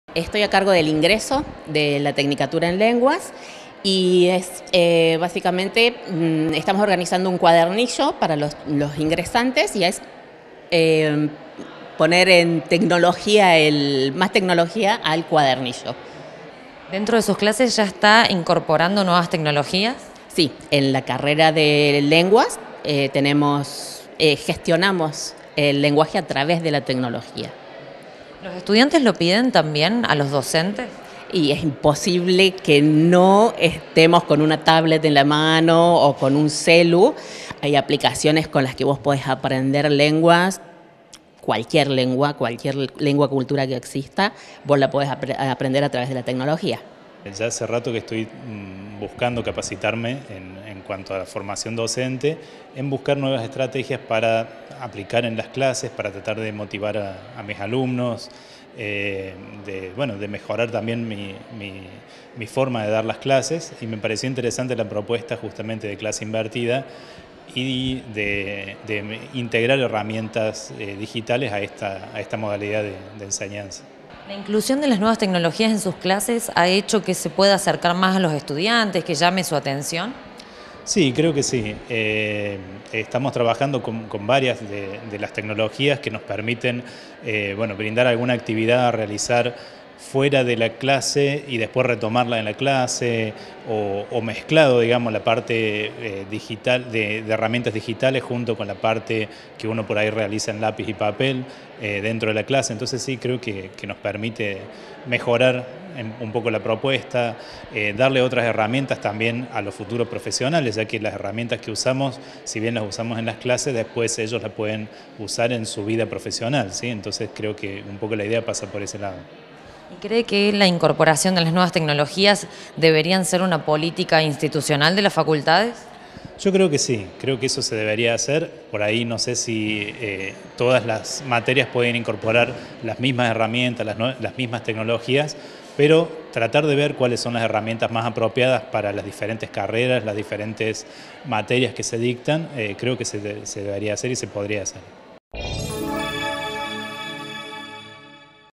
TECNOLOGÍAS DIGITALES - CECILIA IRUSTA Y CRISTIAN DE ANGELO
COMENZÓ EL TALLER QUE PROPONE IMPLEMENTAR “CLASES INVERTIDAS”
Facebook.com/uniriotv
Twitter.com/uniriotv